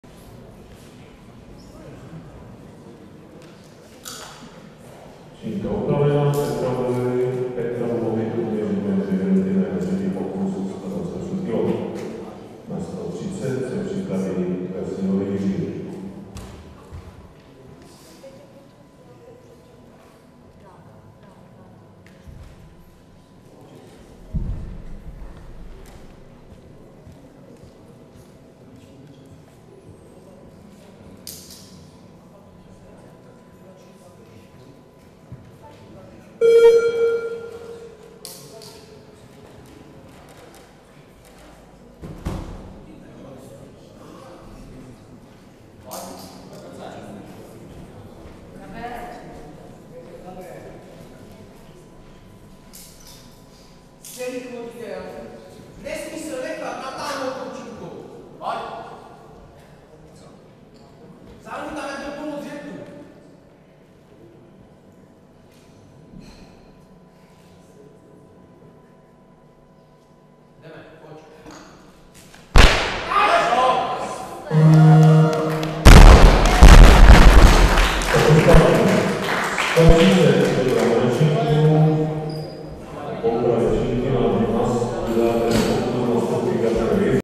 PETROV Petr (1989), TR3, 128 kg
ČEZ liga mužů ve vzpírání družstev - 2. kolo

Místo: Hala policejní školy, Holešov

Federace: ČSV
Pořadatel: TJ Holešov